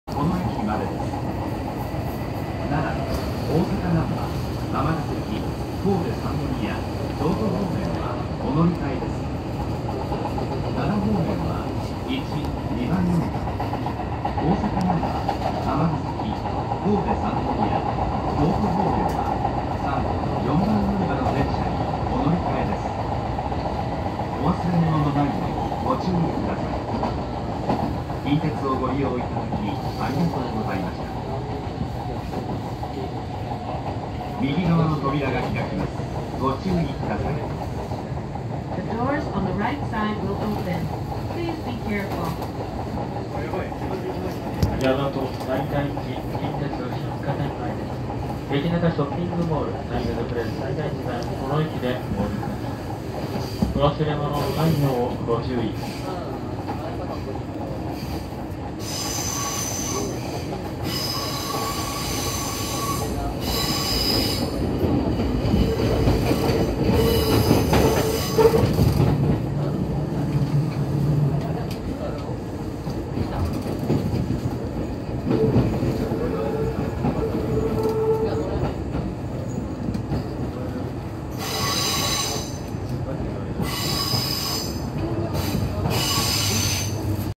近鉄西大寺車庫の様子 橿原神宮前 〜大和西大寺方面の様子 (31)
近鉄西大寺車庫の様子を、大和西大寺行きの普通の中から、撮影しました。
大阪難波行きの普通が、大和西大寺駅に入線する準備をしていました。